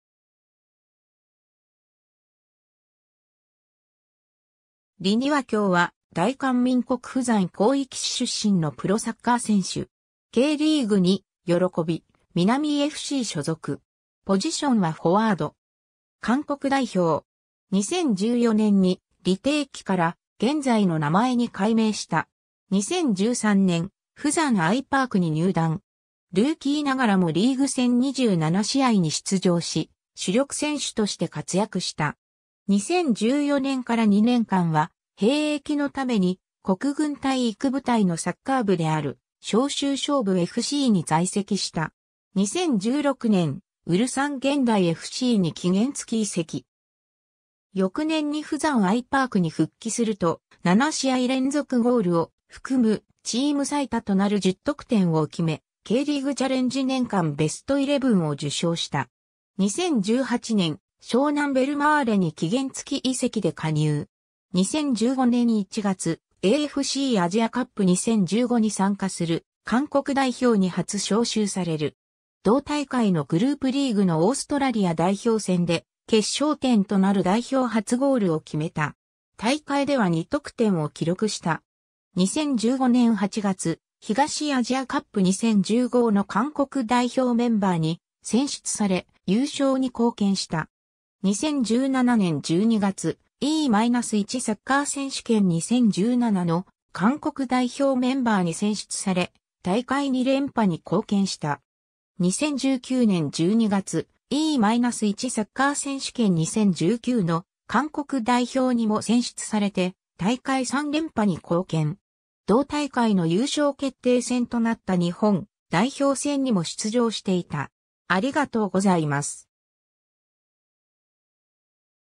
李庭協
李 庭協（イ・ジョンヒョプ、이정협、1991年6月24日 - ）は、大韓民国釜山広域市出身のプロサッカー選手。
Kリーグ2・慶南FC所属。
ポジションはフォワード。
韓国代表。
2014年に李 廷記（イ・ジョンギ、이정기）から現在の名前に改名した。
 2013年、釜山アイパークに入団。
ルーキーながらもリーグ戦27試合に出場し、主力選手として活躍した。
2014年から2年間は兵役のために国軍体育部隊のサッカー部である尚州尚武FCに在籍した。
 2016年、蔚山現代FCに期限付き移籍。
翌年に釜山アイパークに復帰すると、7試合連続ゴールを含むチーム最多となる10得点を決め（韓国人では得点ランキング1位）、Kリーグチャレンジ年間ベストイレブンを受賞した。
 2018年、湘南ベルマーレに期限付き移籍で加入。
 2015年1月、AFCアジアカップ2015に参加する韓国代表に初招集される。
同大会のグループリーグのオーストラリア代表戦で決勝点となる代表初ゴールを決めた。
大会では2得点を記録した。
 2015年8月、東アジアカップ2015の韓国代表メンバーに選出され、優勝に貢献した。
 2017年12月、E-1サッカー選手権2017の韓国代表メンバーに選出され、大会2連覇に貢献した。
 2019年12月、E-1サッカー選手権2019の韓国代表にも選出されて、大会3連覇に貢献。
同大会の優勝決定戦となった日本代表戦にも出場していた。